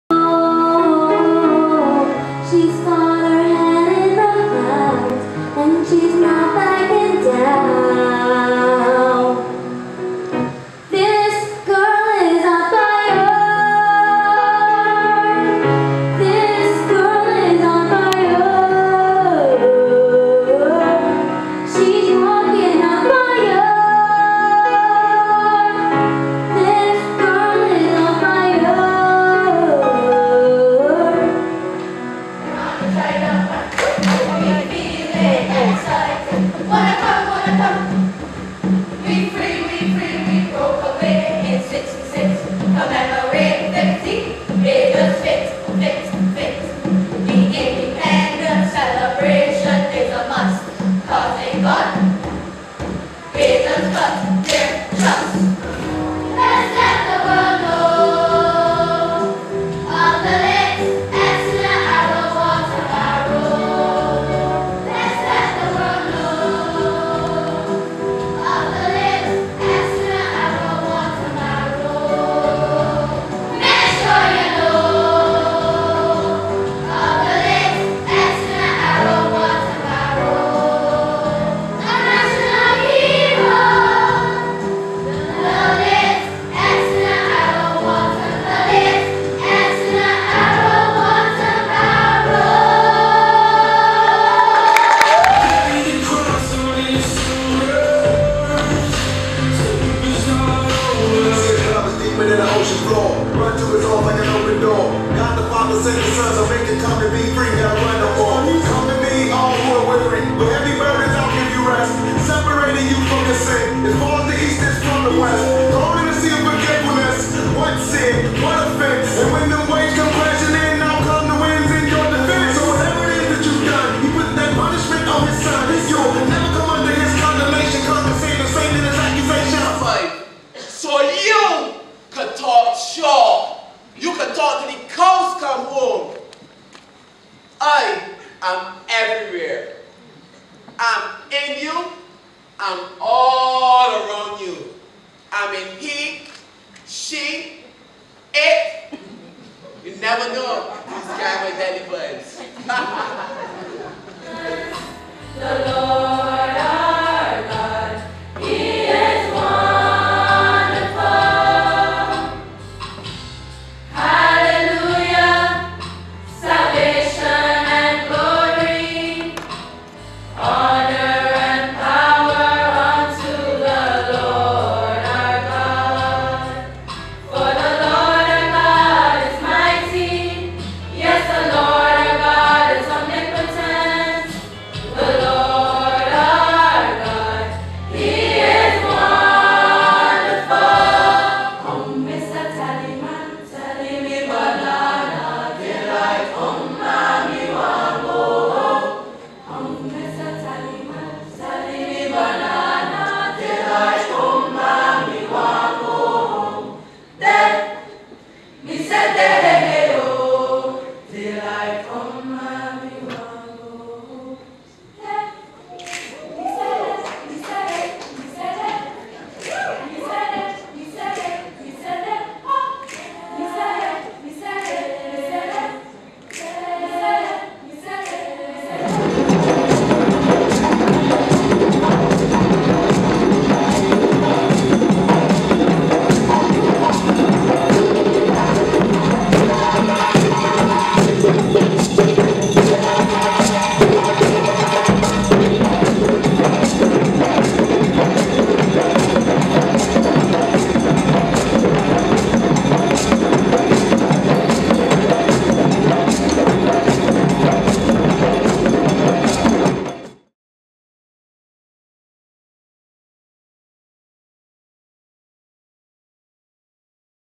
Highlights NIFCA 2016
Highlights from the NIFCA Performing Arts Semi-Finals held at Alexandra School.